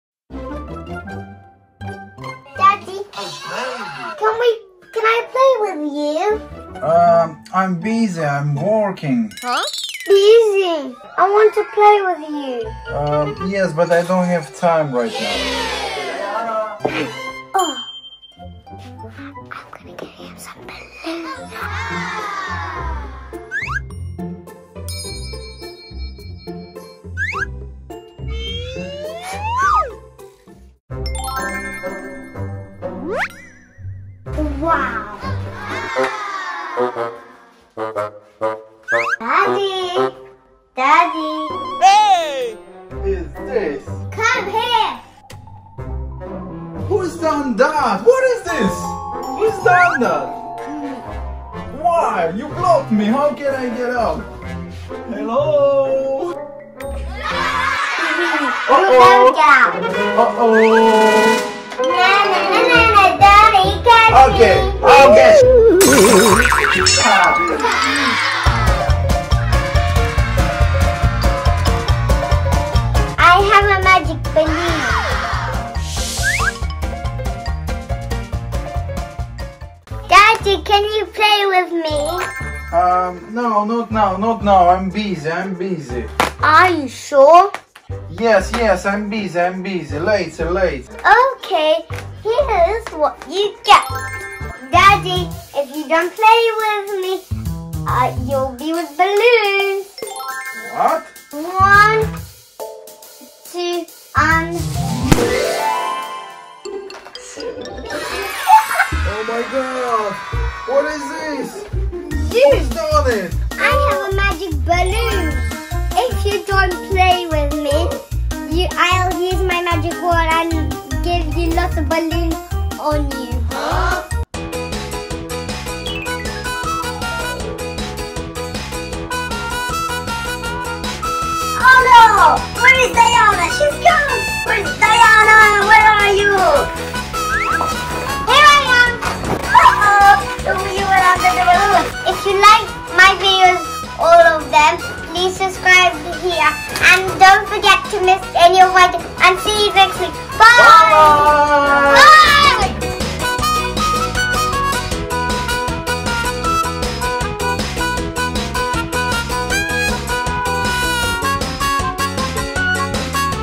DiDi Plays with Funny Balloons
Diana wants to play with Daddy, but he is busy. But she knows how to attract his attention. In this funny pretend video, Diana makes a colorful door with balloons to keep Daddy in his room. Do not miss this funny pretend play story for kids by Diana Springis.

Miss Di